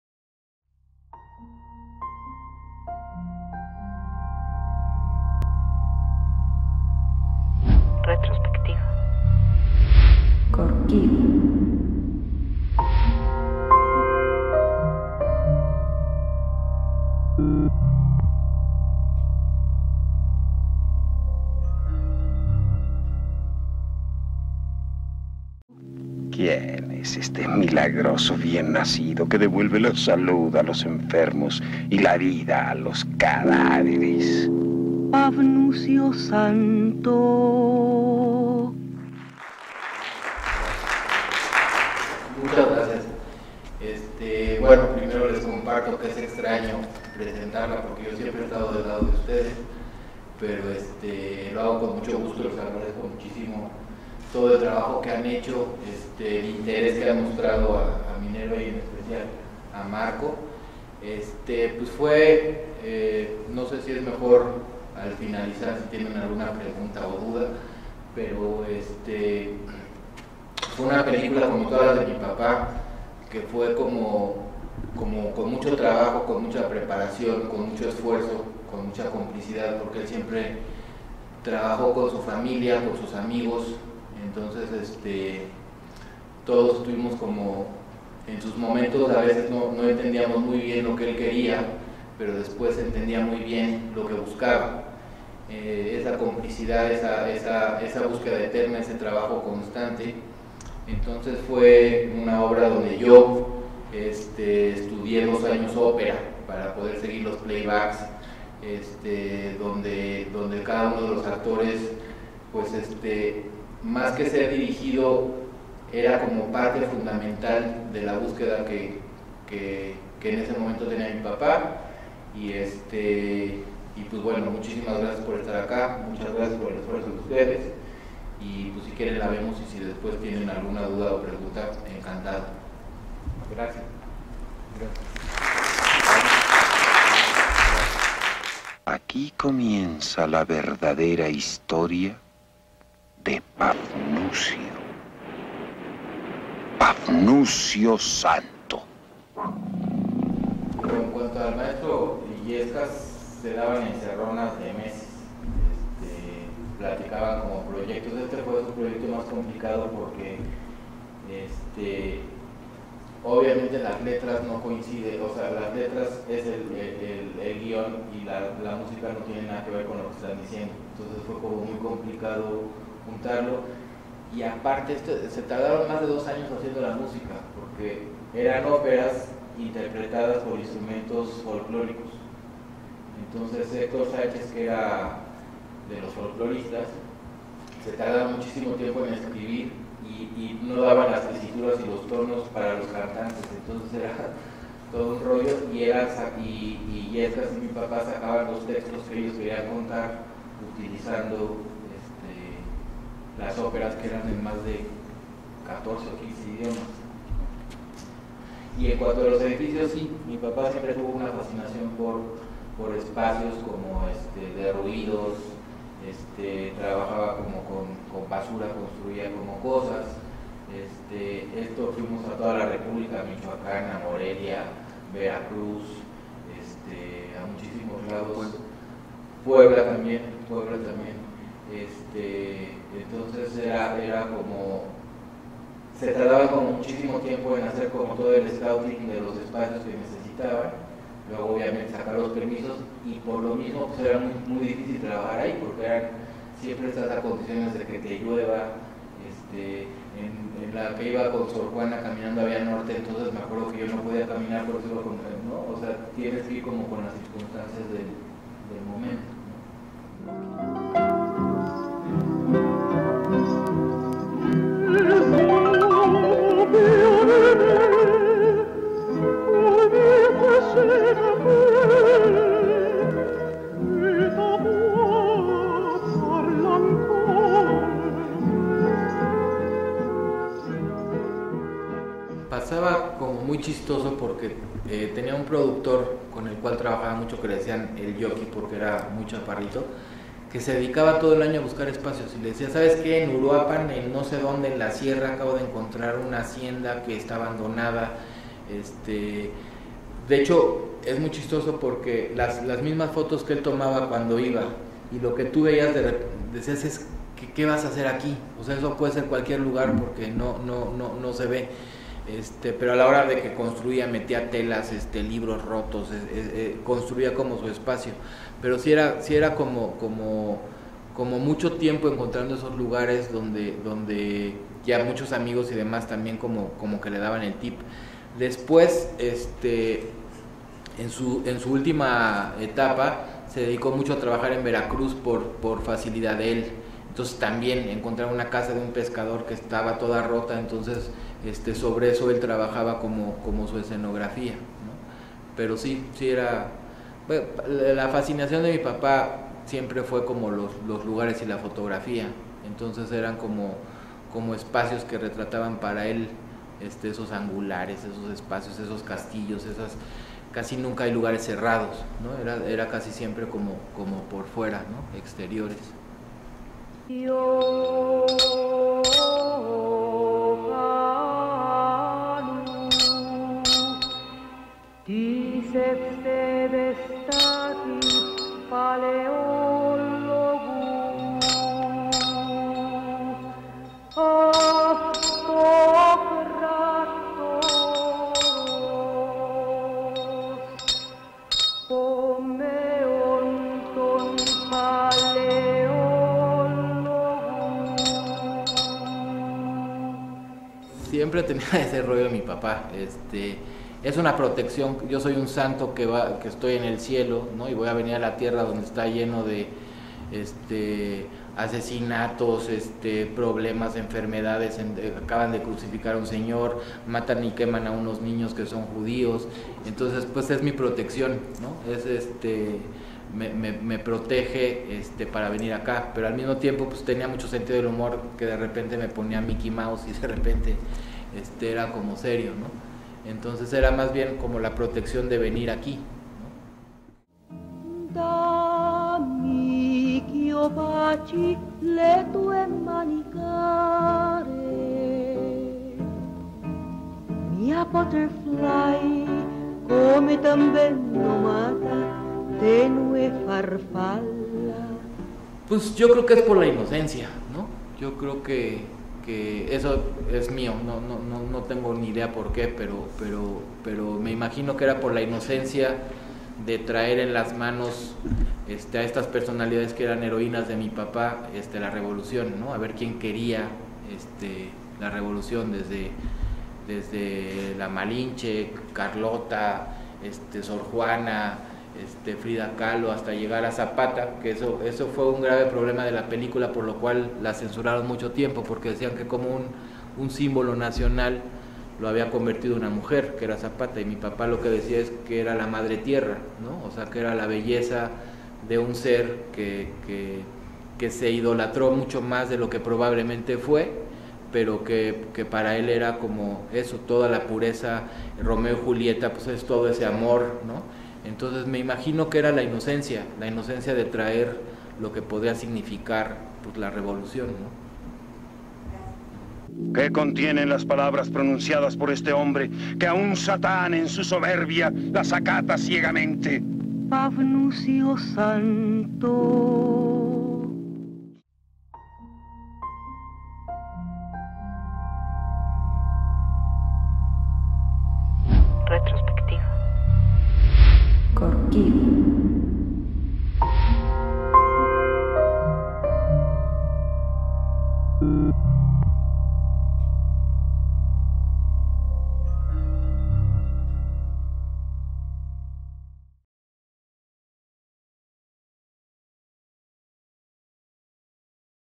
Pablo Corkidi presentando "Pafnucio Santo"
Fragmento de la charla dada por Pablo Corkidi antes y después de la proyección de "Pafnucio Santo" dirigida por Rafael Corkidi dentro de la retrospectiva organizada por CulturaMinerva en Julio 2014.